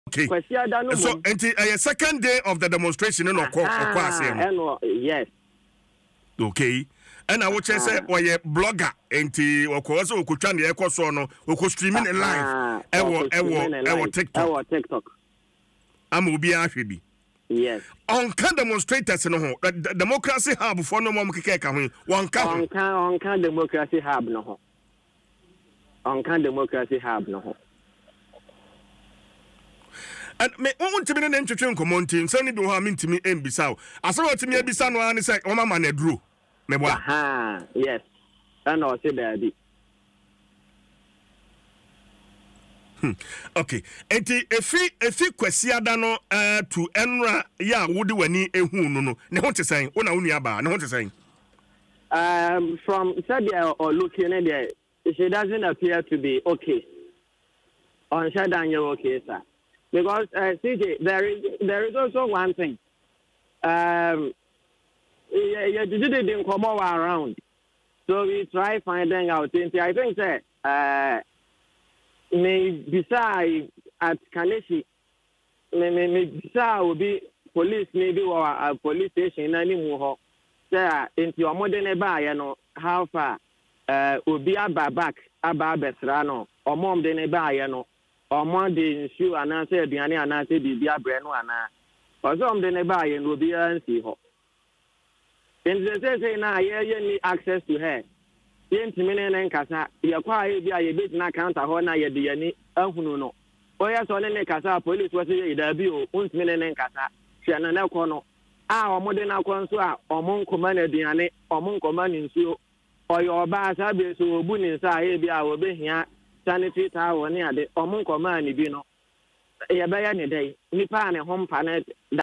DemocracyHubDemo: Protestors have neither been given food nor water since arrest - Akwasi Addo
DemocracyHubDemo: Protestors have neither been given food nor water since arrest - Akwasi Addo ((Brother of a detained protestor)